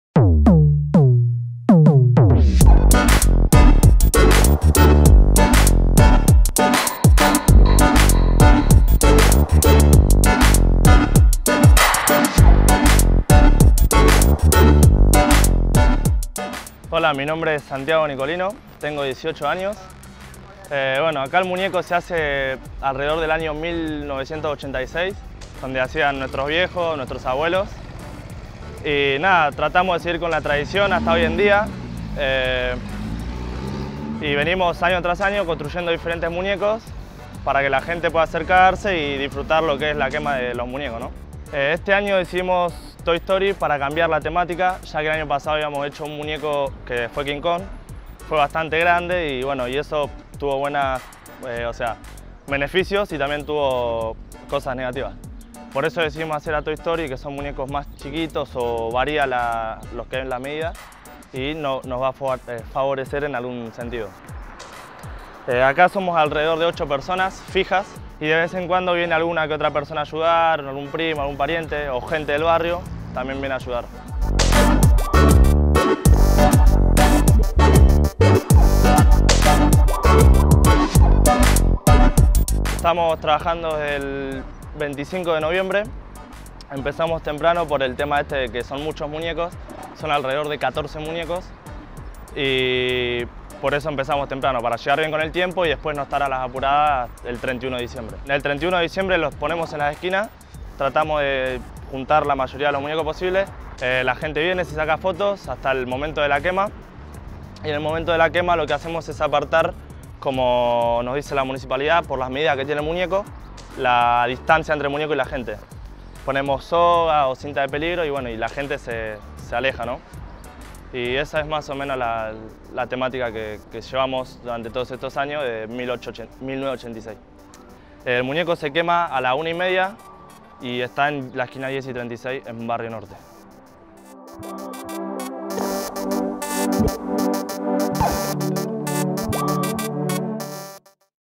MUÑECOS DE FIN DE AÑO | 14 muñecos de Toy Story
Artistas de lo efímero que pasan días sin dormir, sujetos a los cambios del clima y repartiendo guardias para no descuidar ningún detalle de esta tradición que en LaPlata tiene más de sesenta años.

Paran los autos en la calle con un piolin para financiar las obras y piden ayuda a comercios y organizaciones. En varios casos superan los 150 mil pesos. Una locura que no registra antecedentes en el país: “El que no lo vive, no lo entiende” aseguran los platenses

El Grupo 36, que desde 1986 viene dándole vida a la tradición, MONTÓ UN TOTAL DE 14 MUÑECOS del clásico del cine infantil: ToyStory. No faltaron personajes como el Sheriff Woody y BuzzLightyear

0221.com.ar, todos los medios en un sólo lugar 🚀

👀 Leé las últimas noticias de La Plata y la región